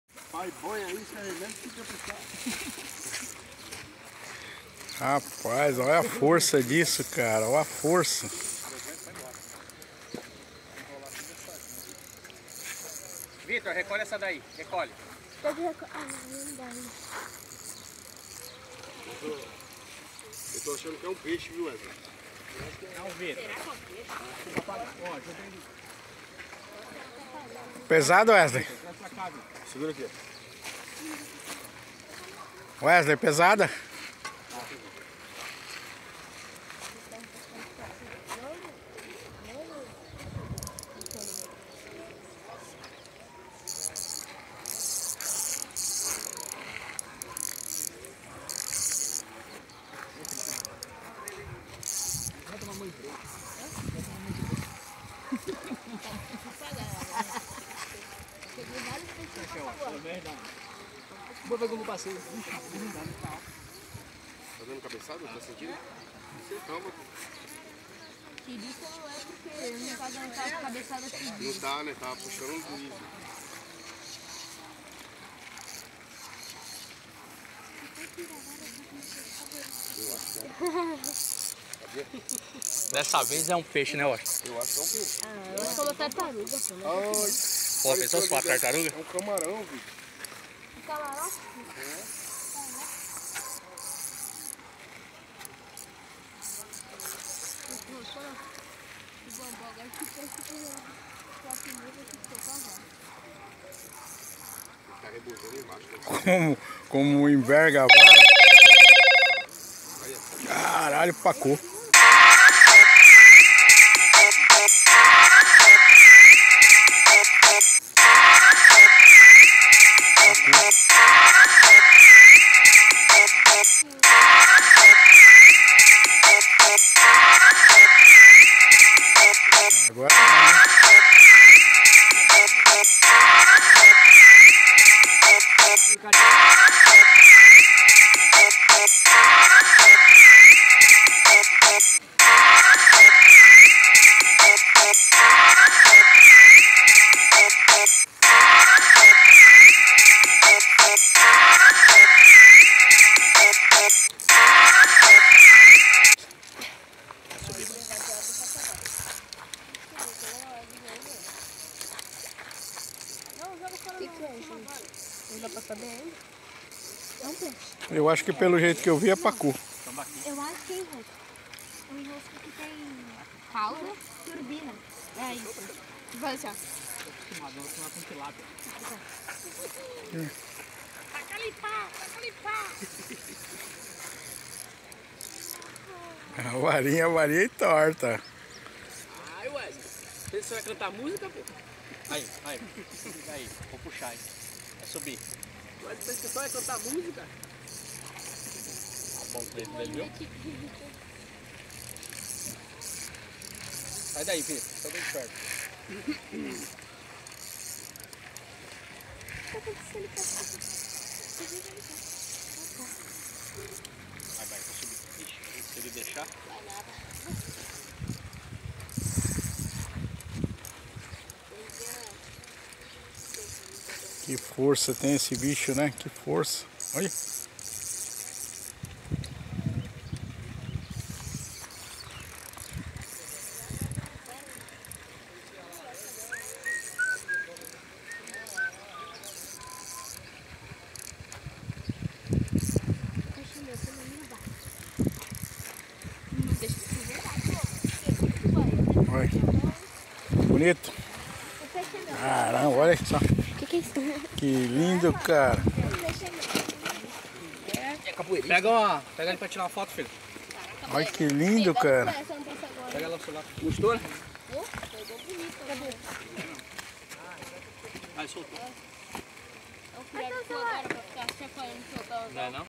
Até que enfim peguei um Tambaqui.
Depois de muito tentar ,saiu meu primeiro tambaqui no Pesqueiro do Horto em São Vicente.  Valeu a pena,não era tão grande ,mas a briga foi legal. Peixe top.